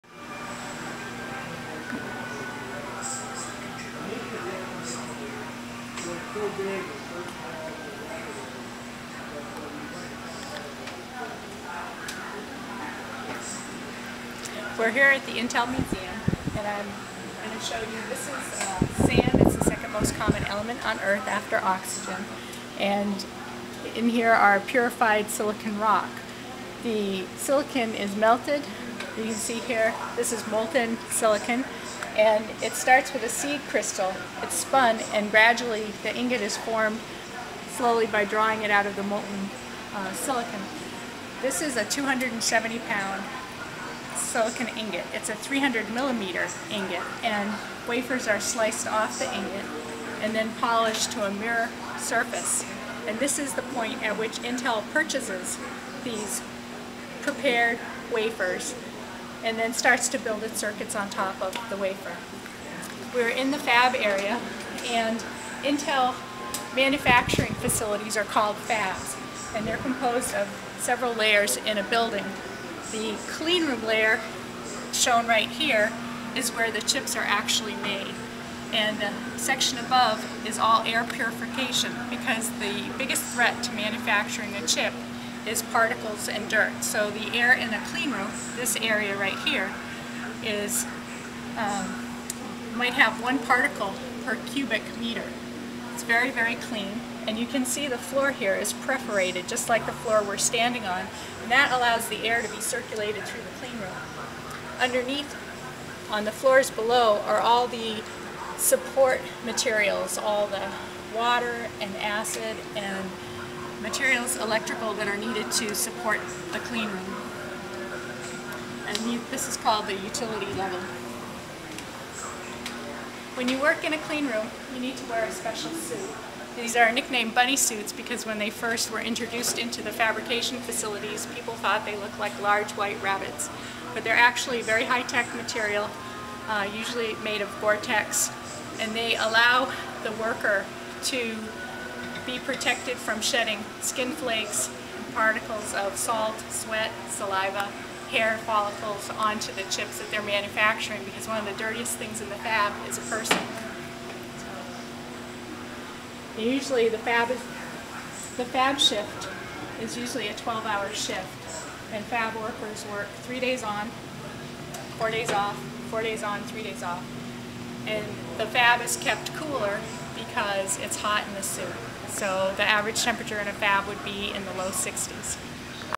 Intel - Silicon becomes a computer 1
A visit to Intel's museum in Santa Clara California